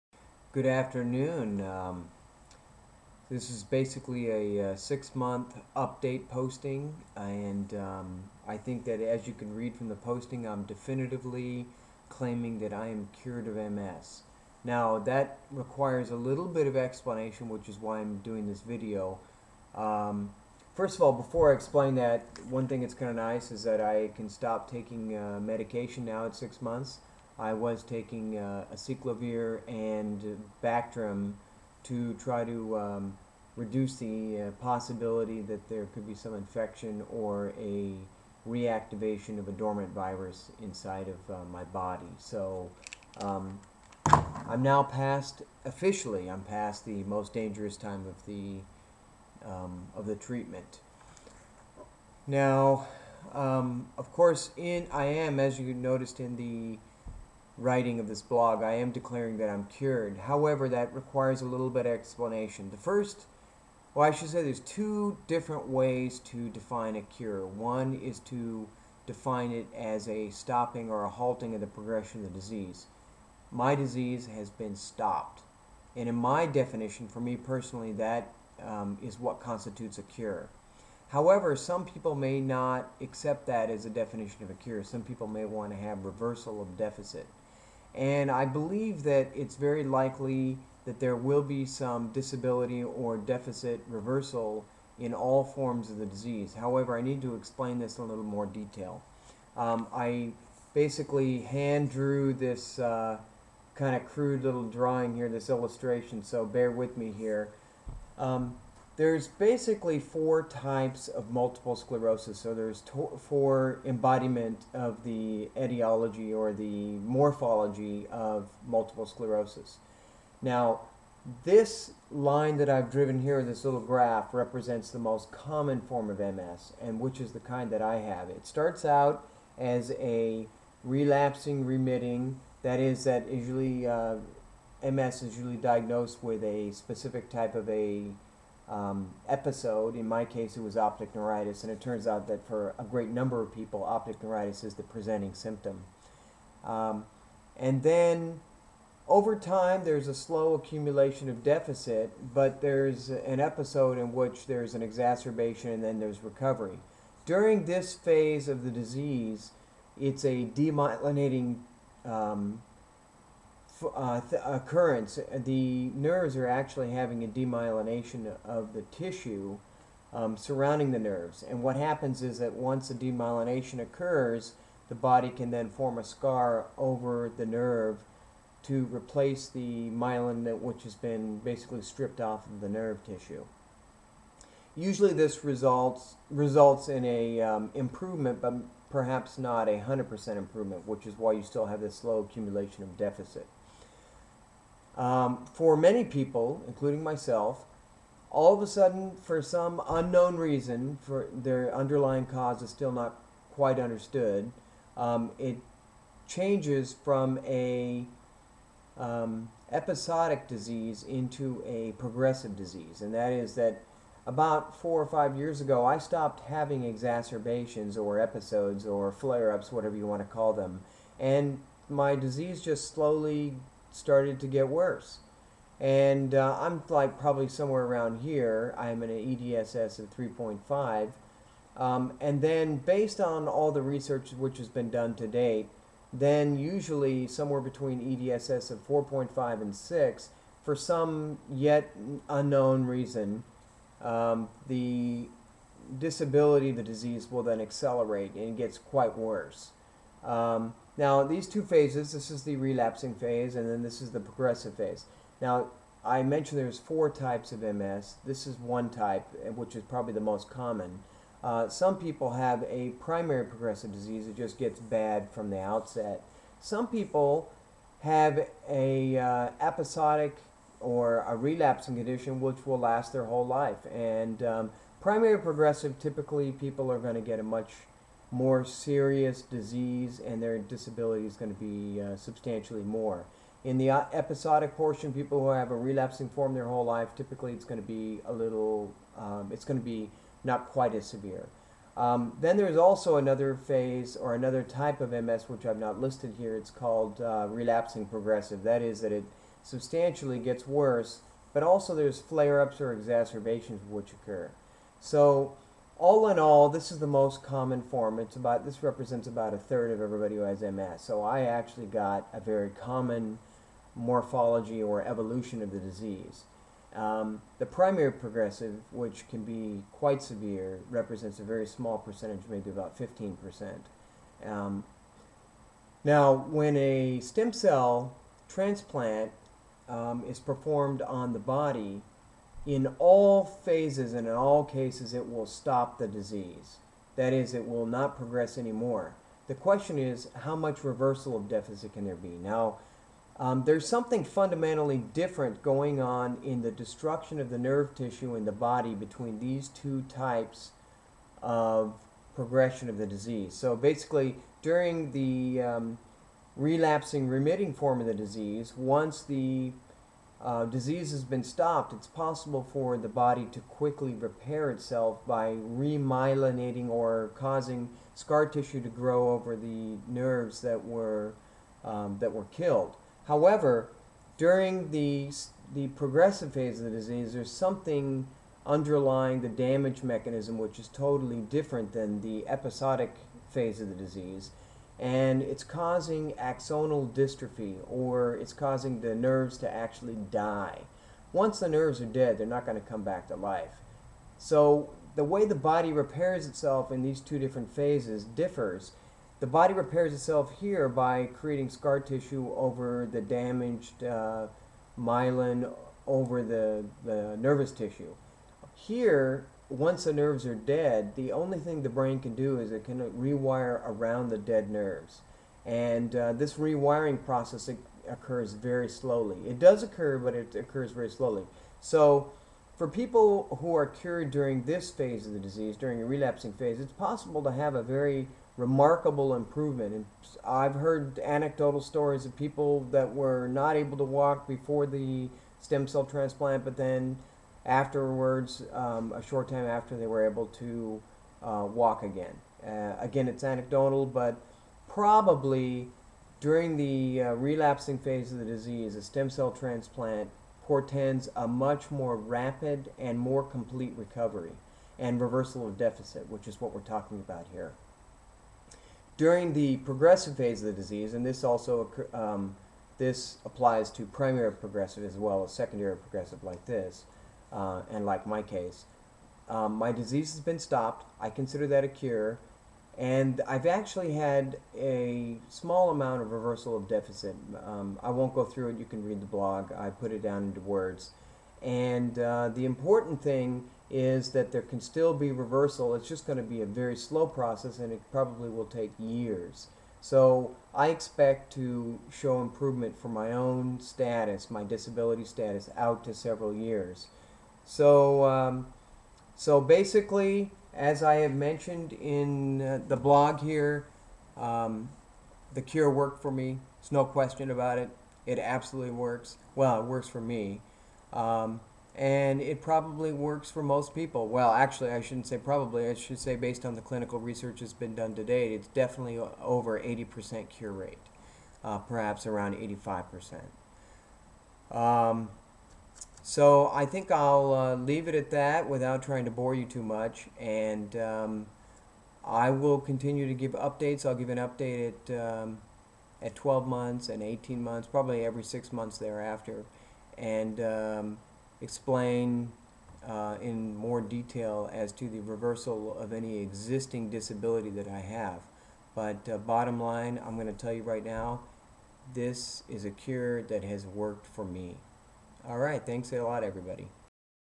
MS Cure - Progression & Recovery Following Stem Cell Transplant
There is only one scientifically established "cure" for Multiple Sclerosis that is available. It is a stem cell transplant (not to be confused with "stem cell therapy").

This video descibes the mechanism of MS recovery & improvement (with deficit reversal) following the cure of a stem cell transplant procedure.